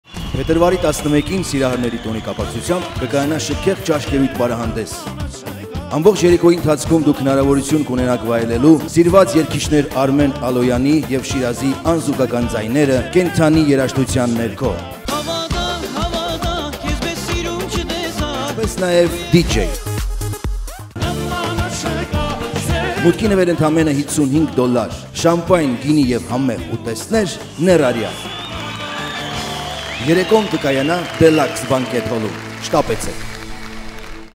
VALENTINES DINNER DANCE PARTY WITH ARMEN ALOYAN & SHIRAZ - FRIDAY, FEB 11TH 2011
OIN US TO CELEBRATE VALENTINE'S DAY WITH:

ARMEN ALOYAN & SHIRAZ
LIVE BAND & DJ MASTER

IT'S GOING TO BE AN UNFORGETTABLE NIGHT AS ARMEN & SHIRAZ ARE GOING TO BE PERFORMING TOGETHER ON STAGE FOR THE FIRST TIME.

IT'S GOING TO BE A NIGHT FILLED WITH NON-STOP MUSIC, DANCING AND MUCH MORE.

HOT & COLD APPETIZERS
FULL DINNER
WINE & CHAMPAGNE

DON'T MISS OUT ON THIS NIGHT...